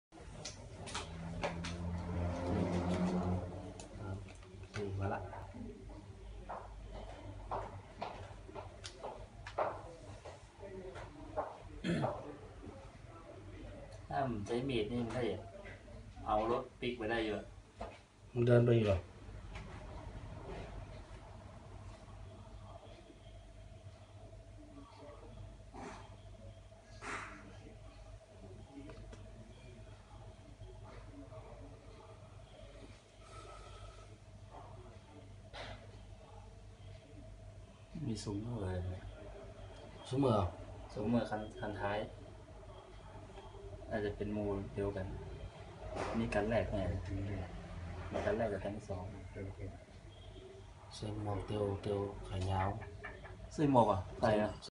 วัยรุ่นถีบรถจักรยานยนต์ชิงมือถือ ที่เชียงใหม่
พ.ศ. 2560 ตำรวจับได้แล้ว